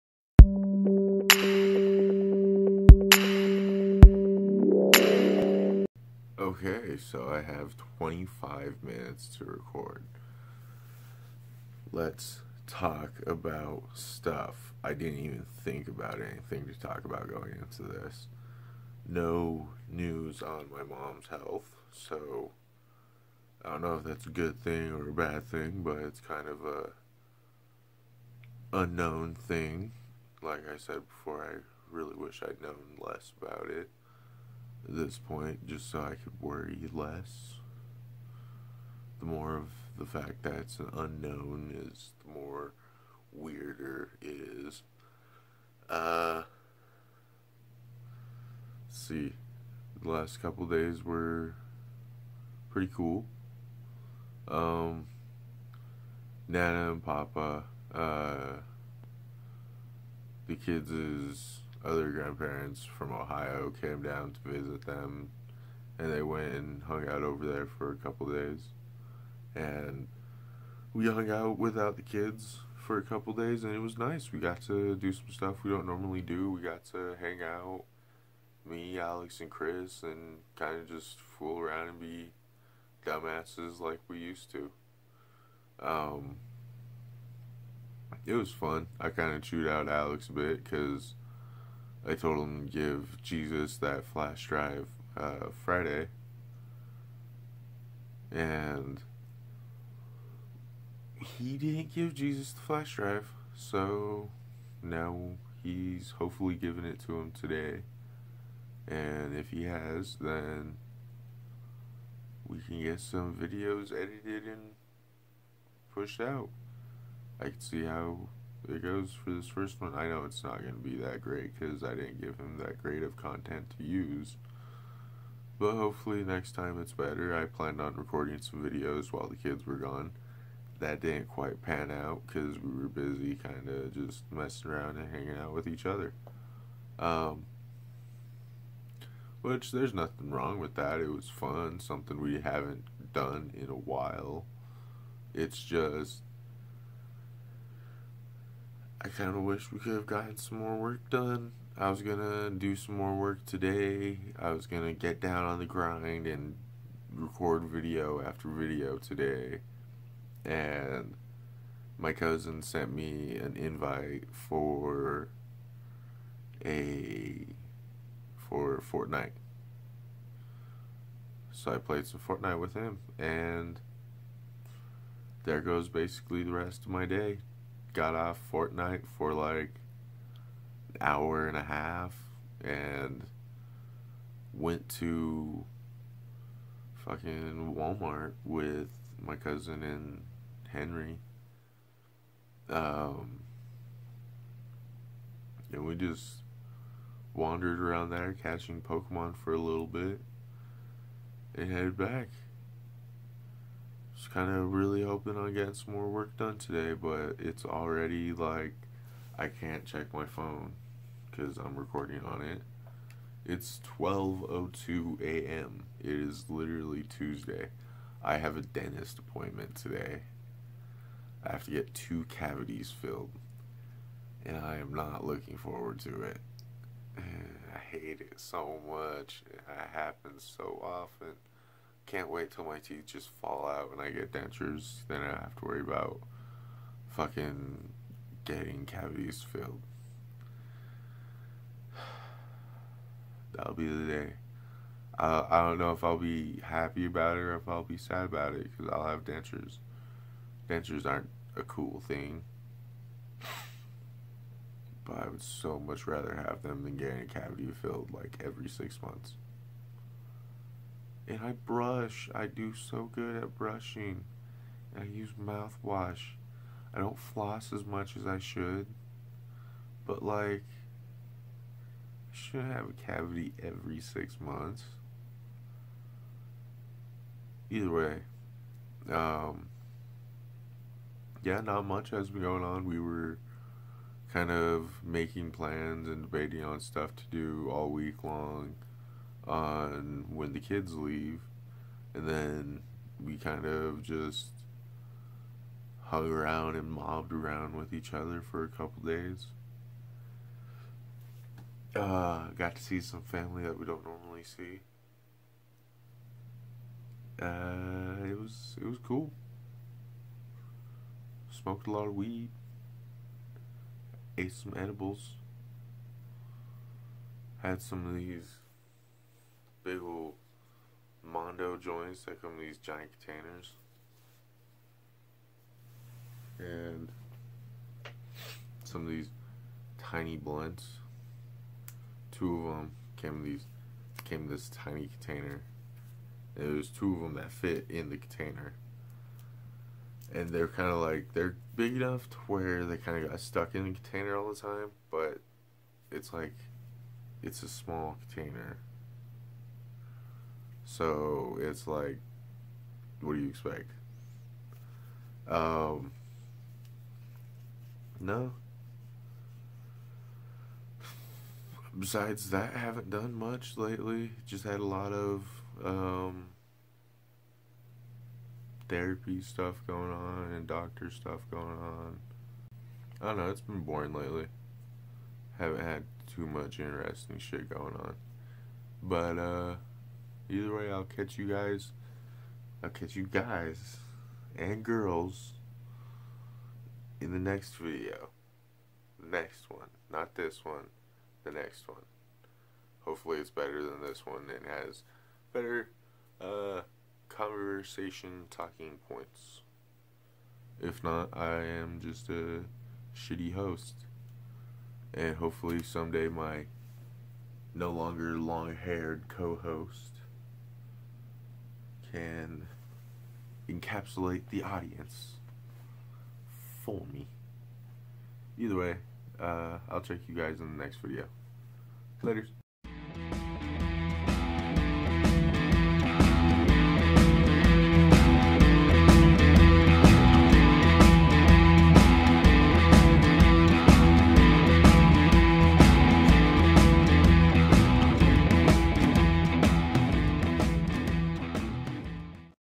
TG of L #39 Time Well Spent!?!
We had a whole weekend to ourselves and spent it not recording videos why because it was fun and family. So ya we suck and sorry that we don't have a dope video for this week.